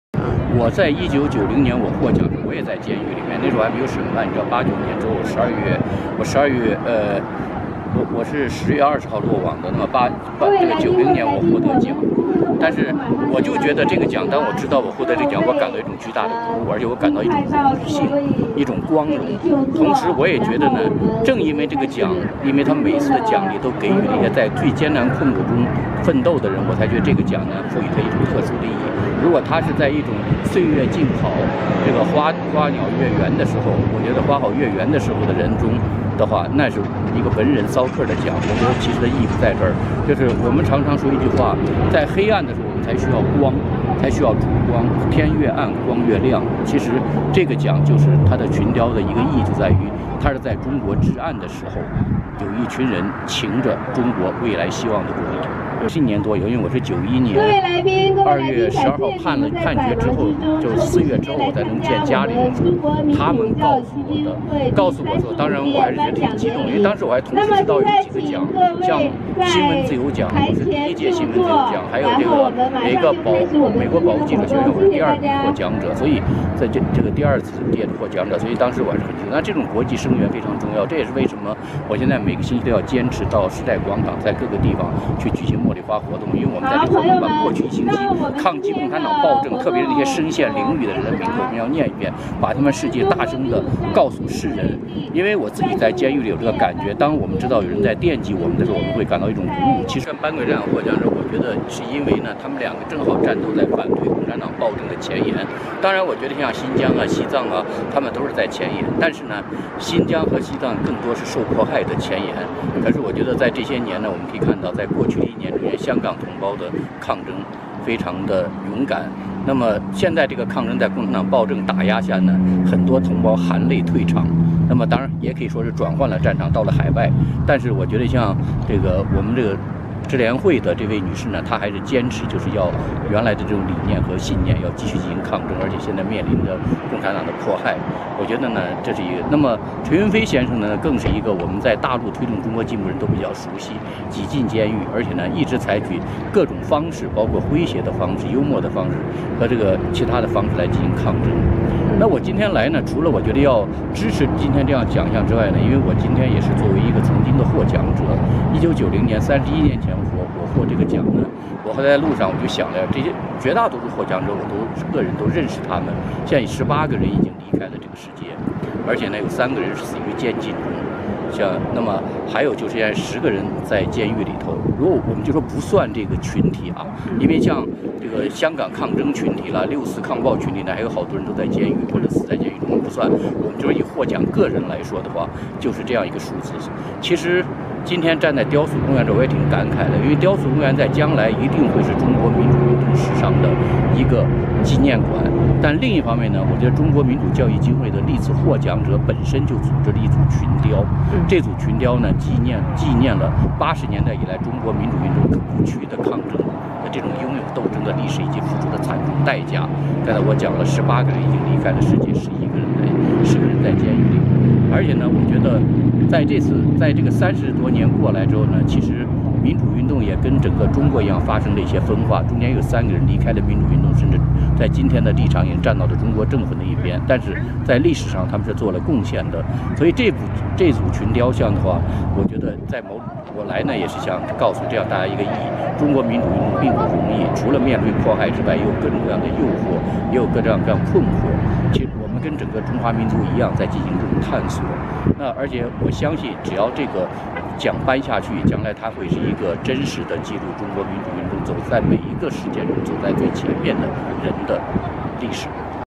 時獲頒#中國傑出民主人士獎 時備受鼓舞遭
王軍濤於1990年獲「中國傑出民主人士獎」時，人仍在中共獄中，當時他被指控為「煽動、組織、指揮反革命暴亂的重要案犯」，得知獲獎後備受鼓舞。他說：「這種國際聲援非常重要，我自己在監獄裡面，知道有人惦記我們，感覺有一種鼓舞。」王軍濤認為：「黑暗的時候才需要光，天越暗的時候，光越亮。它是在中國至暗的時候，有一群人擎著中國未來的火光。」這也是「中國傑出民主人士獎」特殊之處。